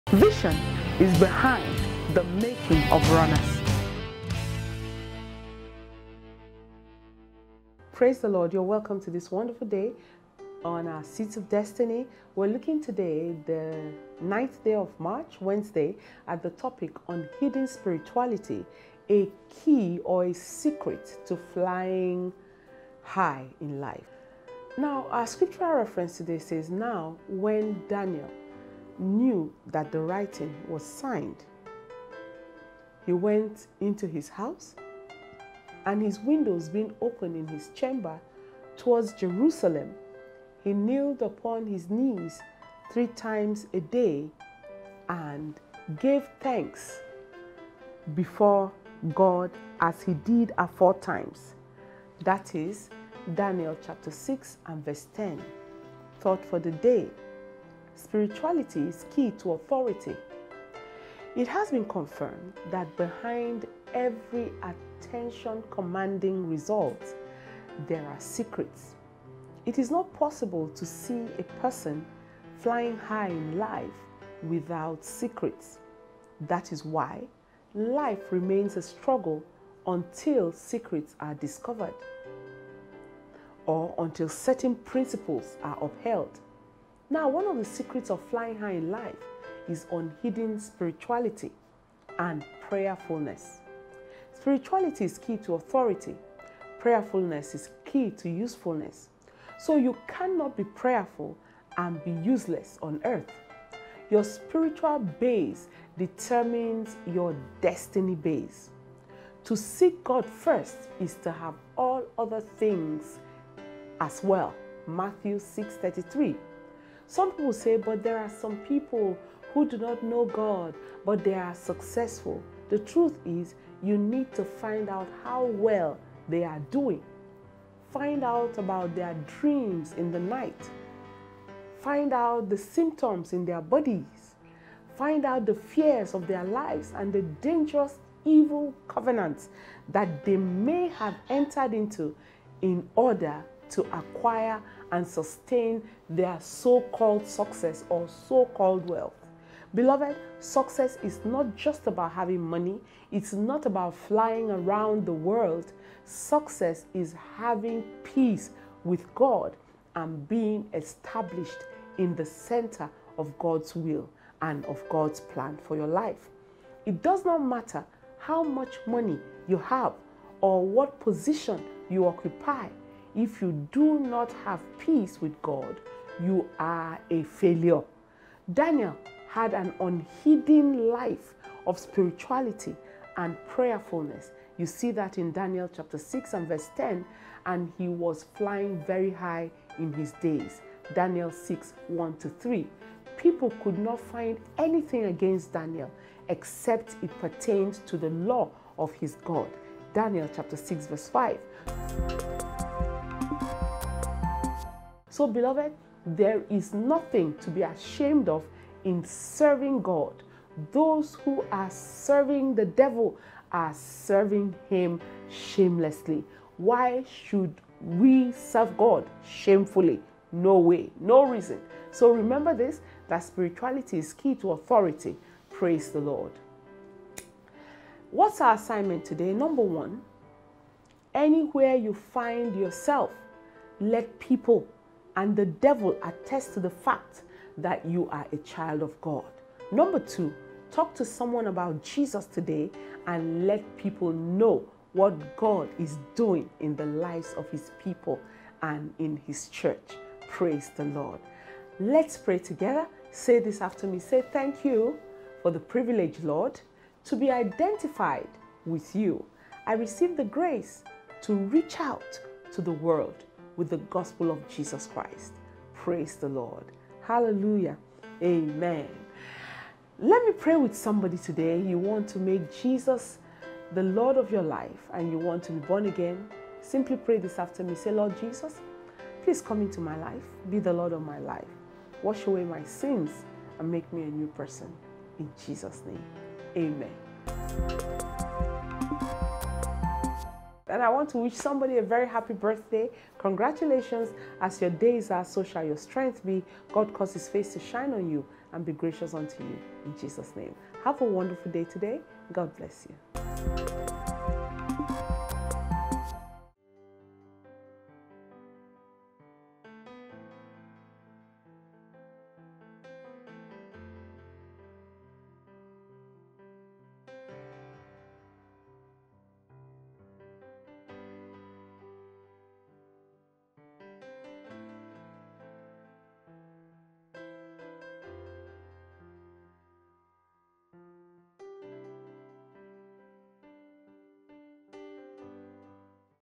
SEEDS OF DESTINY – WEDNESDAY, 09 MARCH, 2022
TOPIC: UNHIDDEN SPIRITUALITY – A SECRET TO FLYING HIGH IN LIFE
Spirituality is key to authority. Prayerfulness is key to usefulness. You cannot be prayerful and be useless on earth. Click to WATCH today's

SCRIPTURE: Now when Daniel knew that the writing was signed, he went into his house; and his windows being open in his chamber toward Jerusalem, he kneeled upon his knees three times a day, and prayed, and gave thanks before his God, as he did aforetime. Daniel 6:10

THOUGHT FOR THE DAY: Spirituality is key to authority.

It has been confirmed that behind every attention-commanding result, there are secrets. It is not possible to see a person flying high in life without secrets. That is why life remains a struggle until secrets are discovered or certain principles are upheld.

Now, one of the secrets of flying high in life is unhidden spirituality and prayerfulness.

Spirituality is key to authority. Prayerfulness is key to usefulness. You cannot be prayerful and be useless on earth. Your spiritual base determines your destiny base.

To seek God first is to have all other things (Matthew 6:33).
Some people say, “But there are people who do not know God but are successful”. The truth is, you need to find out how well they are doing. Find out about their dreams in the night, the symptoms in their bodies, the fears of their lives, and the dangerous evil covenants they may have entered into in order to acquire and sustain the so-called success.

Beloved, success is not just about having money; it is not about flying around the world. Success is having peace with God and being established in the Centre of God’s Will and Plan for your life. It does not matter how much money you have or what position you occupy, if you do not have peace with God, you are a failure.

Daniel had an unhidden life of spirituality and prayerfulness (Daniel 6:10). And he was flying very high in his days (Daniel 6:1-3). People could not find anything against Daniel except it pertained to the Law of his God (Daniel 6:5).

Beloved, there is nothing to be ashamed of in serving God. Those who are serving the devil are serving him shamelessly. Why should we serve God shamefully?

Remember this: Spirituality is key to authority.

ASSIGNMENT(S):
1. Anywhere you find yourself, let people and the devil attest to the fact that you are a child of God.
2. Talk to someone about Jesus today; and let people know what God is doing in the lives of His people in His Church.

PRAYER: Lord, thank You for the privilege to be identified with You. I receive the grace to reach out to the world with the Gospel of Jesus Christ, Lord in Jesus’ Name.

FOR FURTHER UNDERSTANDING, GET THIS MESSAGE: SECRETS OF HIGHER FLIERS.

DAILY BIBLE READING: 1 Kings 5-7.

QUOTE: Every believer must come to the realization that their office is their parish; their table is their pulpit and their work is their ministry. Culled from “21 FOOLISH THINGS PEOPLE DO” by Dr Paul Enenche.

AMAZING FACT: No matter how badly fingerprints are damaged, they will always grow back in their original pattern. What a wonder of God!

PROPHETIC DECLARATION/WORD: Receive fresh fire for unhidden spiritual intensity in Jesus’ Name.